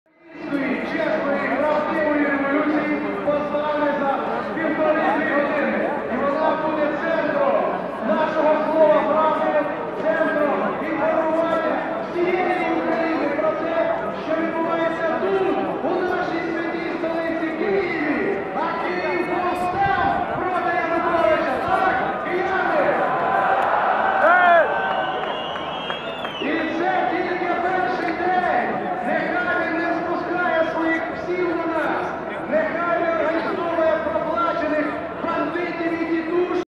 Майдан Незалежності станом на 20:00
Майдан Незалежності станом на 8 годину вечора (1 грудня).